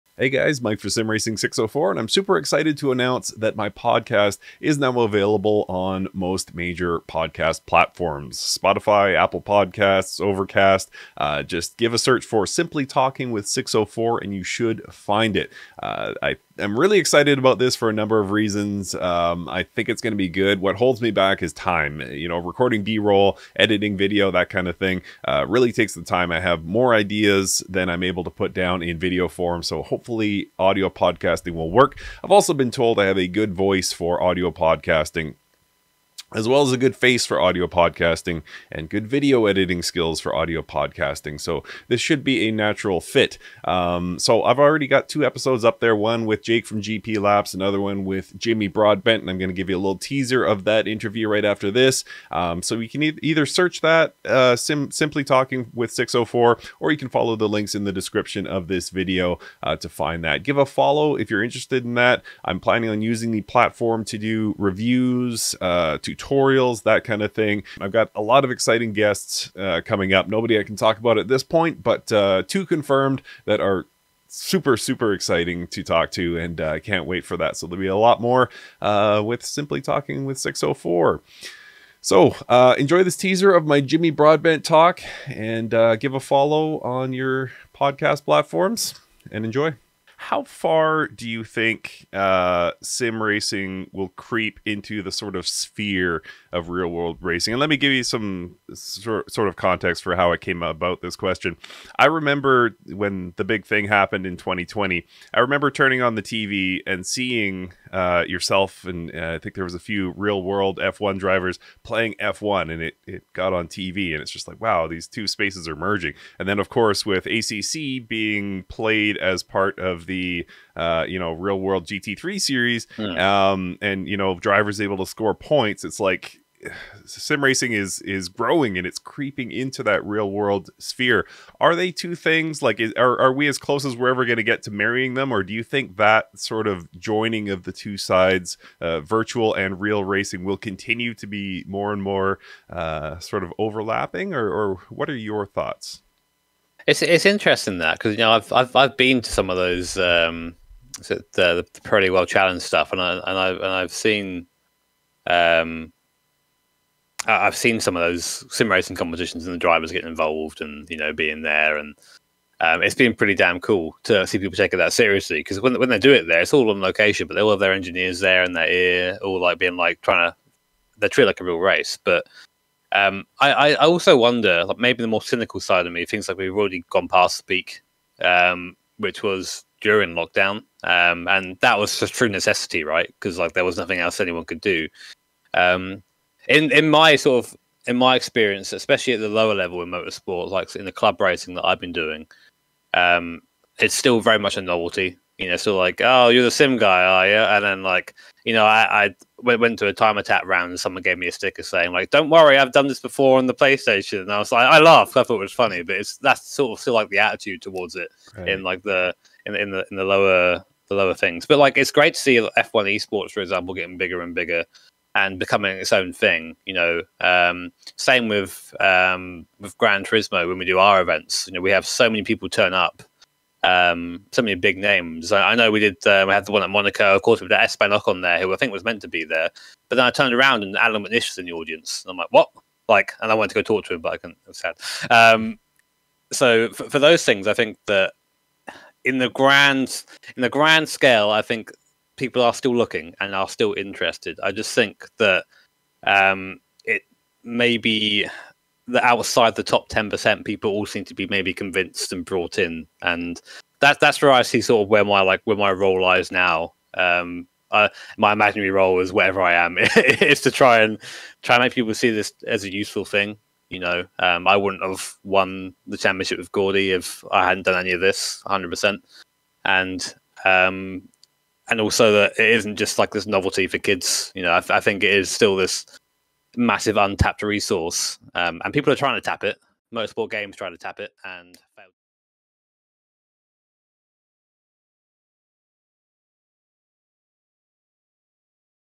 SIMply Talking Podcast is Now Available on Streaming Services!
I'm happy to announce that my new podcast, SIMply Talking with 604, is now available across most major streaming platforms.  I've already been fortunate enough to record with Jake from GPLaps and Jimmy Broadbent, which you'll get a teaser of in this video.

If you're using another podcast player, try searching "SIMply Talking with 604"

Video Card (MSi Radeon RX 6800 XT GAMING X TRIO 16G)